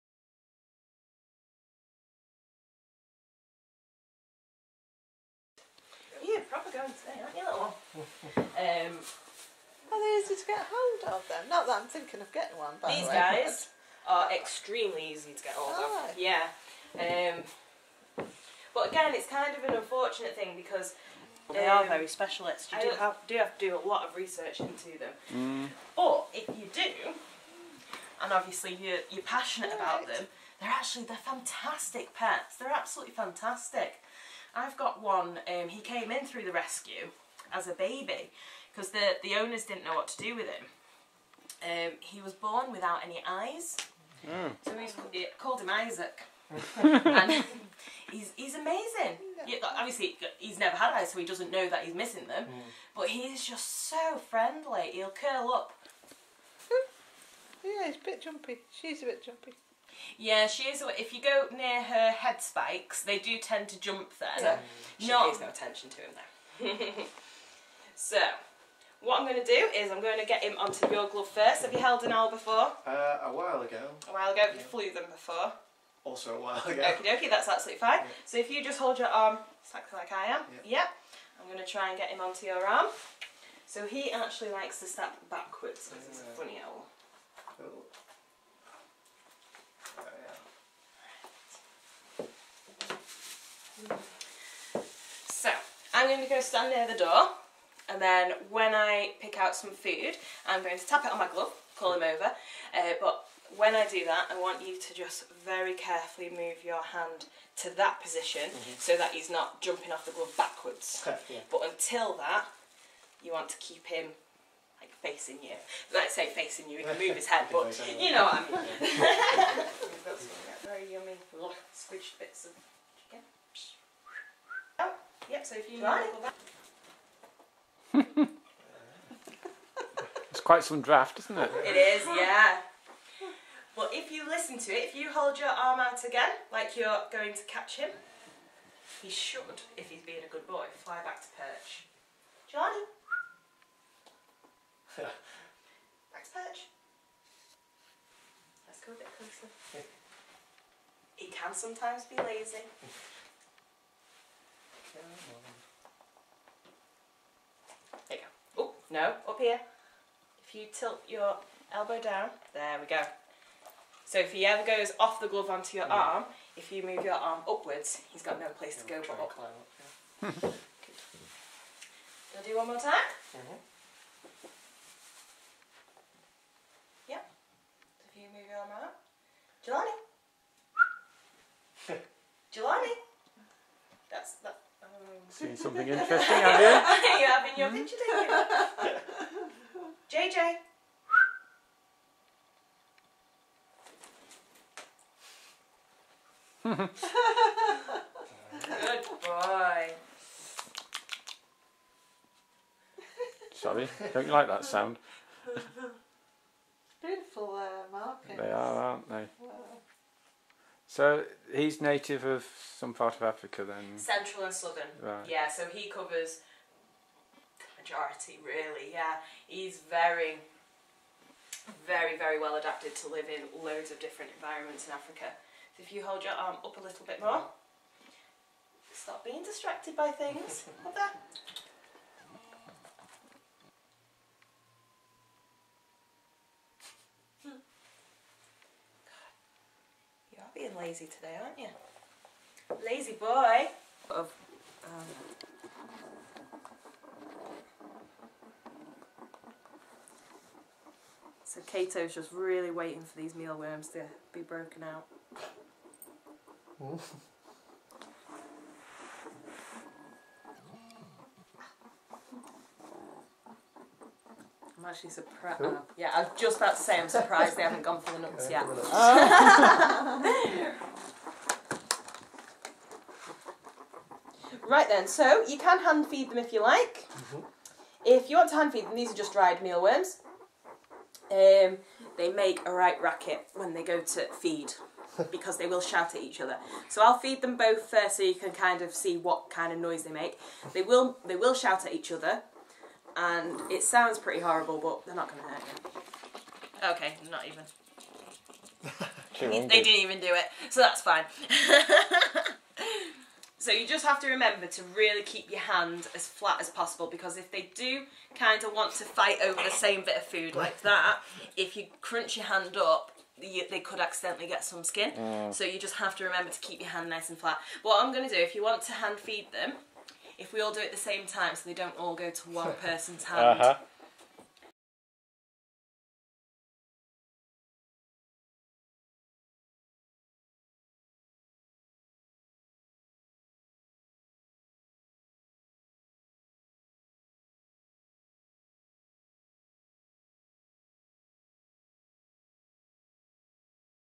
A visit from Zoo2U
Christmas 2015 we had a visit from Zoo2U at our home.